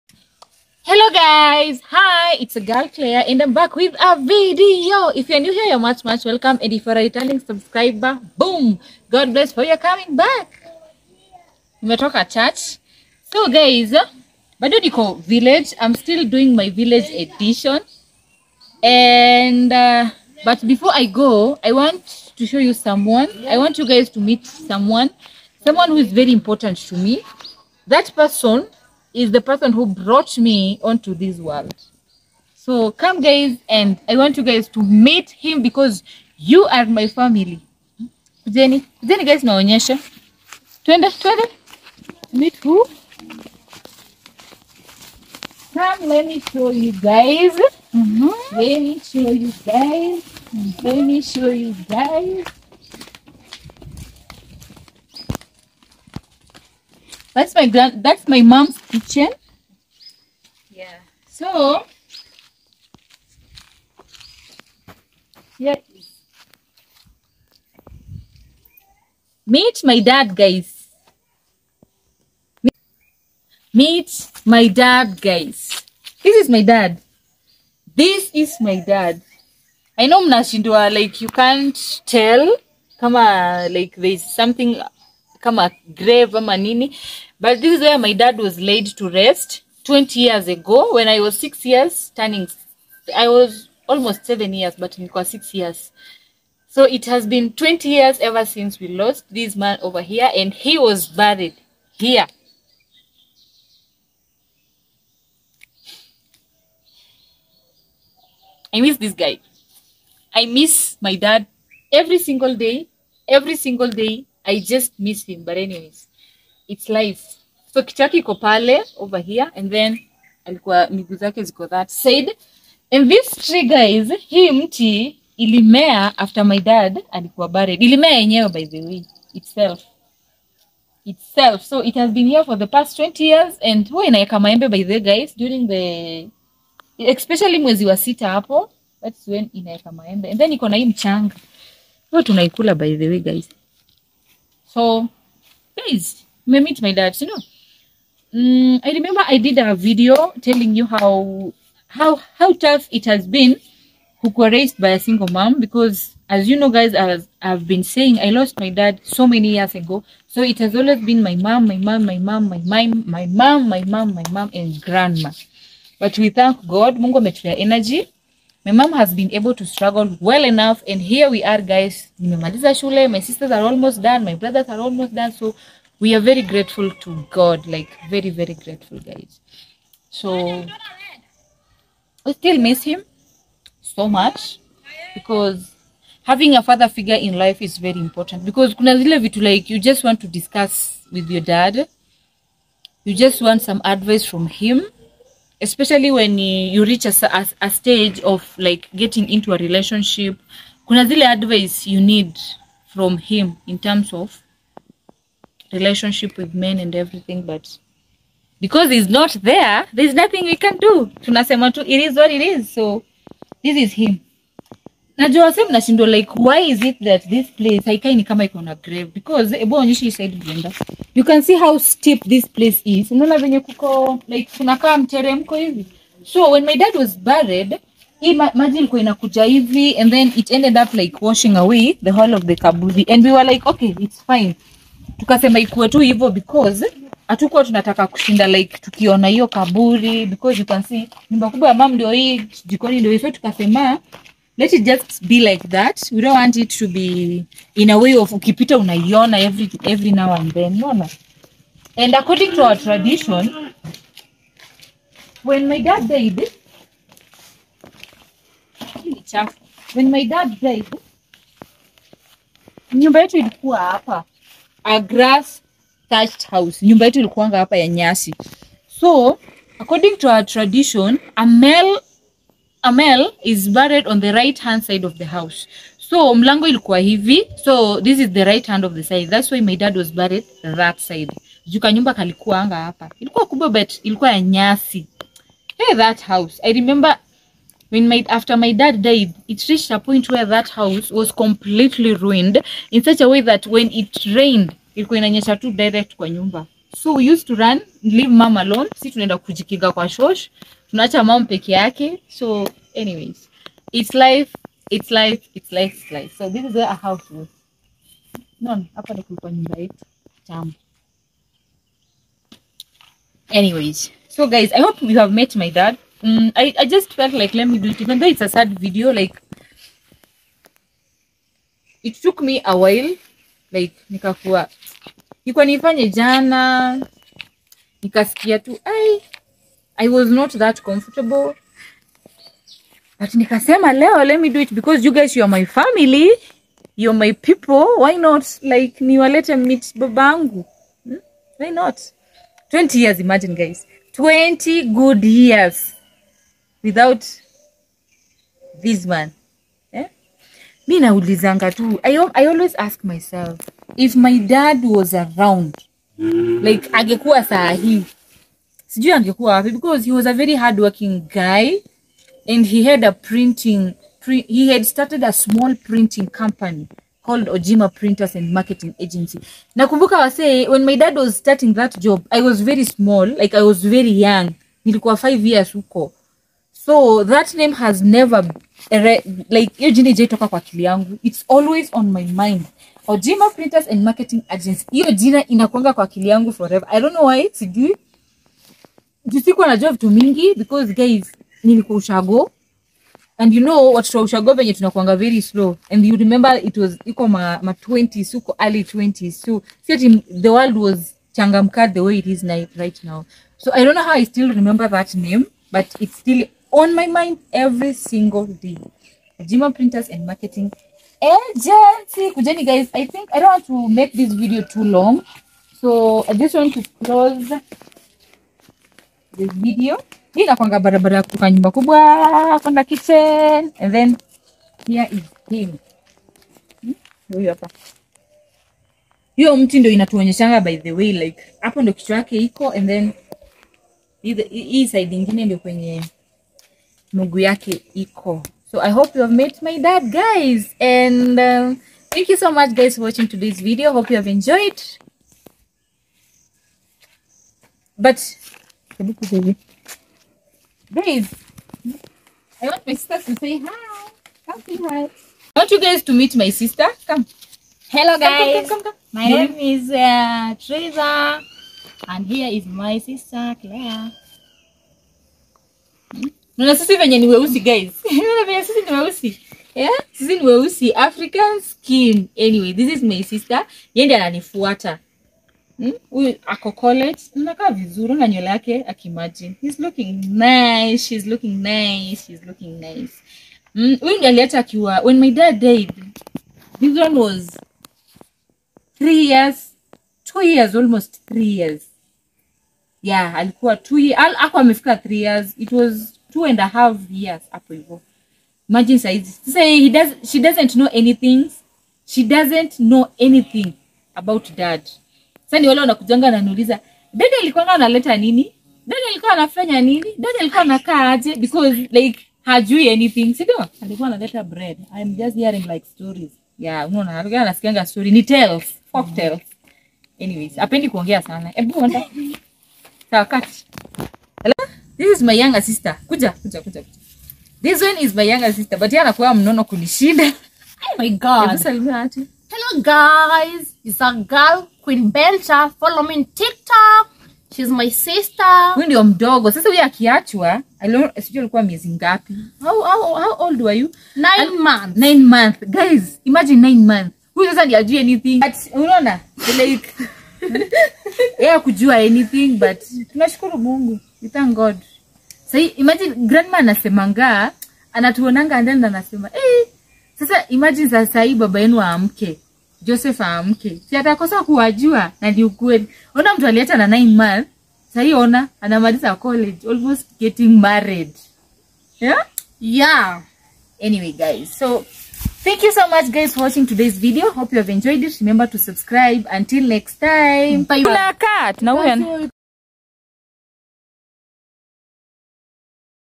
EMOTIONAL;Visiting my DAD'S grave after 20 Years of his death😭😭
Hello my beautiful people,this video is so so emotional as l visited my dads grave. We lost him in year 2023 and it was never easy and still is not easy but its all Gods plan.